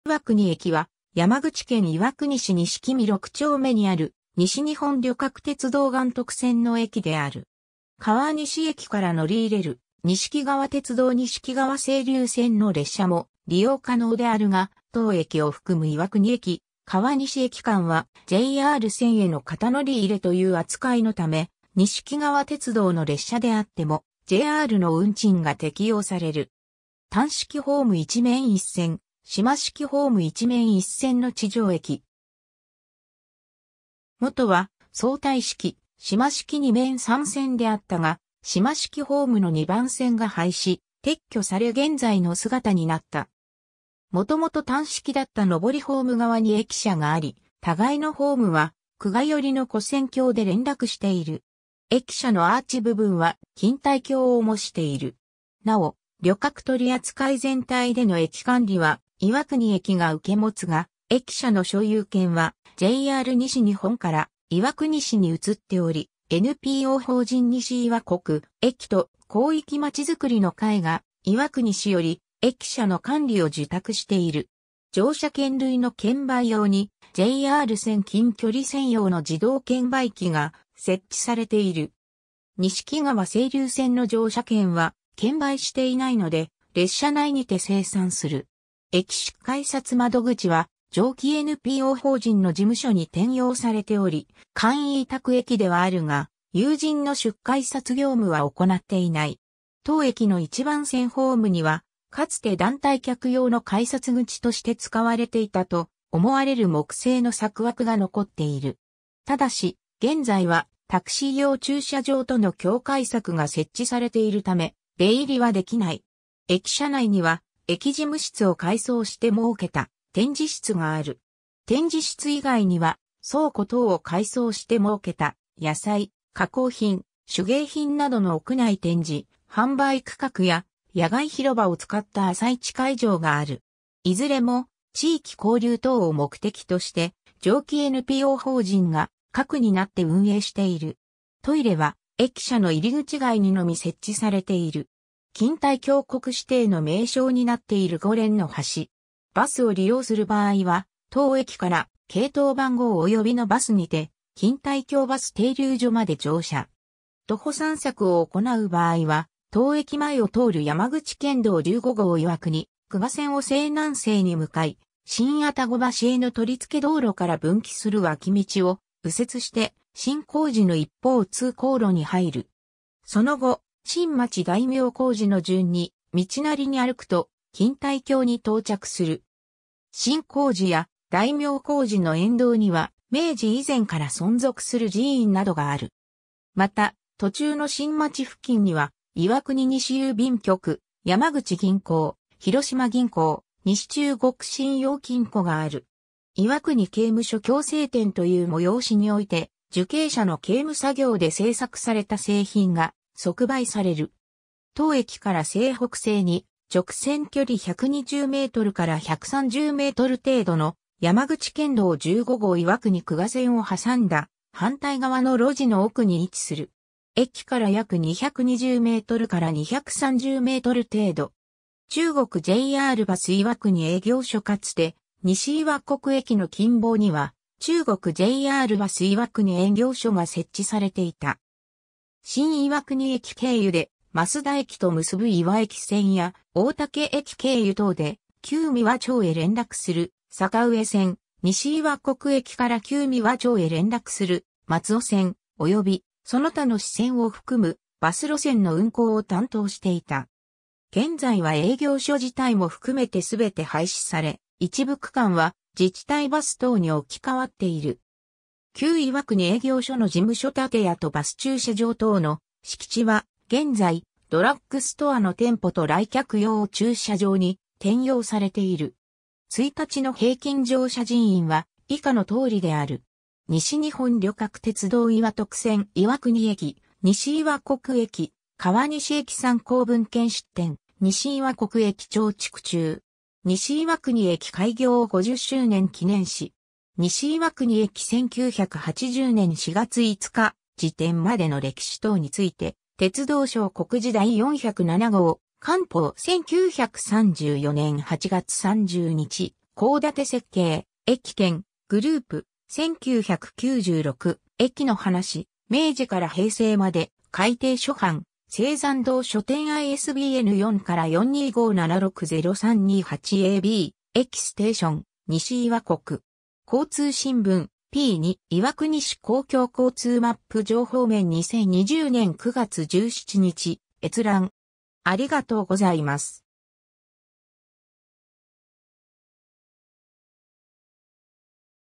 西岩国駅
別の角度から 西岩国駅（にしいわくにえき）は、山口県岩国市錦見（にしみ）六丁目にある、西日本旅客鉄道（JR西日本）岩徳線の駅である。
 川西駅から乗り入れる錦川鉄道錦川清流線の列車も利用可能であるが、当駅を含む岩国駅 - 川西駅間はJR線への片乗り入れという扱いのため、錦川鉄道の列車であってもJRの運賃が適用される。
 単式ホーム1面1線、島式ホーム1面1線の地上駅。
もとは相対式・島式2面3線であったが、島式ホームの2番線が廃止、撤去され現在の姿になった。
元々単式だった上りホーム側に駅舎があり、互いのホームは玖珂寄りの跨線橋で連絡している。
 駅舎のアーチ部分は、錦帯橋を模している。
なお、旅客取扱全体での駅管理は岩国駅が受け持つが、駅舎の所有権はJR西日本から岩国市に移っており、NPO法人西岩国・駅と広域まちづくりの会が岩国市より駅舎の管理を受託している。
 乗車券類の券売用に、JR線近距離専用の自動券売機が設置されている。
錦川清流線の乗車券は券売していないので、列車内にて精算する。
駅出改札窓口は、上記NPO法人の事務所に転用されており、簡易委託駅ではあるが有人の出改札業務は行っていない（自動券売機の設置前は窓口で乗車券の販売を行っていた。券売機の売り上げの一部をNPO法人の収入とする形で窓口業務の代わりとしている。この手法は北陸本線の河毛駅などでも見られる）。
 当駅の1番線ホームには、かつて団体客用の改札口として使われていたと思われる木製の柵枠が残っている。
ただし、現在はタクシー用駐車場との境界柵が設置されている為、出入りはできない。
 駅舎内には、駅事務室を改装して設けた展示室がある。
展示室以外には、倉庫等を改装して設けた、野菜・加工品・手芸品などの屋内展示・販売区画（旬彩館）や、野外広場を使った朝市会場（土曜朝市）がある。
いずれも、地域交流等を目的として上記NPO法人が核になって運営している。
 トイレは、駅舎の入口外にのみ設置されている。
 錦帯橋 国指定の名勝になっている五連の橋。
 バスを利用する場合は、当駅から系統番号（いちすけ号）およびのバスにて、「錦帯橋」バス停留所まで乗車。
 徒歩散策を行なう場合は、当駅前を通る山口県道15号岩国玖珂線を西南西に向かい、新愛宕橋への取り付け道路から分岐する脇道を右折して新小路の一方通行路に入る（錦見5丁目と錦見6丁目の境界付近）。
その後、新町（岩国2丁目 - 3丁目付近）→大名小路（錦帯橋通り）の順に道なりに歩くと、錦帯橋に到着する（全行程：約1.8km）。
 新小路や大名小路の沿道には、明治以前から存続する寺院などがある。
また、途中の新町付近には、岩国西郵便局、山口銀行、広島銀行、西中国信用金庫（いずれもATM設置）がある。
 岩国刑務所 矯正展という催しにおいて、受刑者の刑務作業で製作された製品が即売される。
 当駅から西北西に直線距離120m - 130m程度の、山口県道15号岩国玖珂線を挟んだ反対側の路地の奥に位置する。
駅から約220m - 230m程度。
 中国JRバス（旧国鉄バス）岩国営業所 かつて、西岩国駅の近傍（錦見町6丁目14。現在の県道岩国玖珂線と国道2号に挟まれた区画）には、中国JRバス岩国営業所（後に、周防営業所岩国支所に格下げ）が設置されていた。
新岩国駅経由で益田駅と結ぶ岩益線や、大竹駅経由等で旧美和町へ連絡する坂上線、西岩国駅から旧美和町へ連絡する松尾線、および、その他の支線を含むバス路線の運行を担当していた。
現在は営業所（および関連する六日市支所等）自体も含めて全て廃止され、一部区間は自治体バス等に置き換わっている。
 旧岩国営業所の事務所建屋とバス駐車場等の敷地は、現在、ドラッグストア（ウォンツ）の店舗と来客用駐車場に転用されている。
 1日の平均乗車人員は以下の通りである。
 西日本旅客鉄道 ■岩徳線（錦川鉄道錦川清流線の列車含む） 岩国駅 - 西岩国駅 - 川西駅 参考文献 出典 ^ 西岩国駅長　竹中 (1980-6). 西岩国駅開業50周年記念誌. （国鉄）西岩国駅 1980年4月5日時点までの歴史等について ^ 「鉄道省告示第407号」『官報』1934年8月30日（国立国会図書館デジタルコレクション） ^ 交建設計・駅研グループ、1996、『駅のはなし 明治から平成...